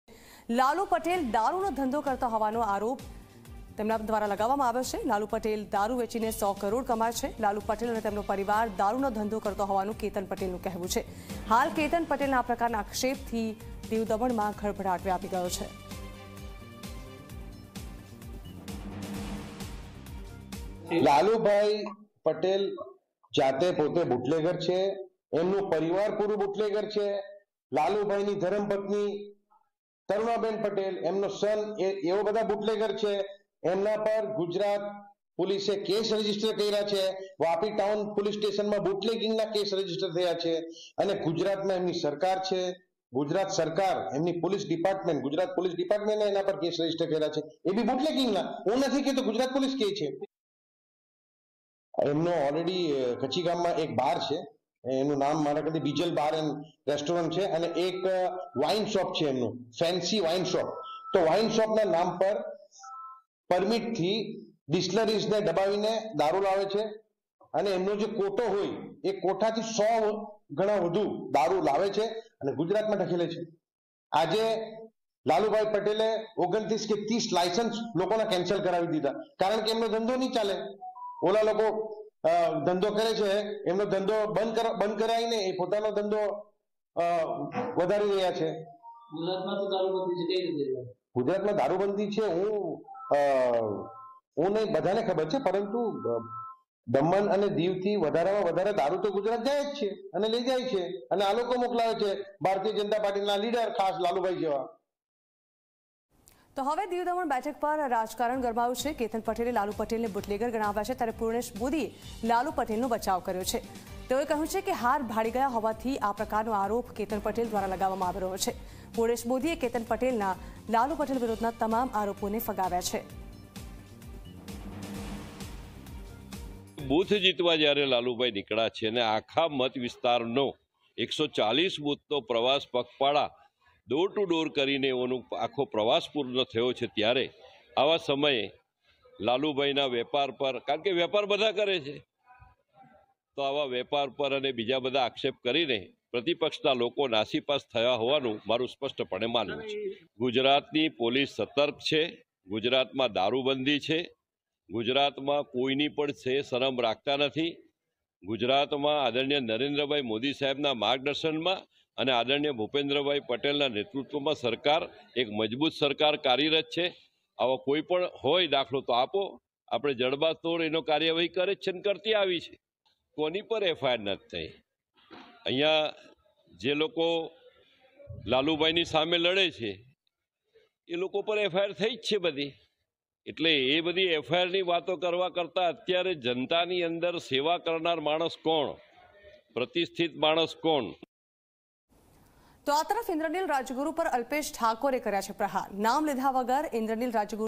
"હાર ભાળી ગયા હોવાથી આરોપ લગાવ્યો": Purnesh Modiએ Lalu Patel પર Ketan Patelના આરોપોને ફગાવ્યા
દીવ દમણ બેઠક પર ગરમાયું રાજકારણ ---
કેતન પટેલે લાલુ પટેલને બુટલેગર ગણાવ્યા ---
પૂર્ણેશ મોદીએ લાલુ પટેલનો કર્યો બચાવ ---
હાર ભાળી ગયા હોવાથી આરોપ લગાવ્યો: પૂર્ણેશ મોદી ---
પૂર્ણેશ મોદીએ કેતન પટેલના આરોપોને ફગાવ્યા ---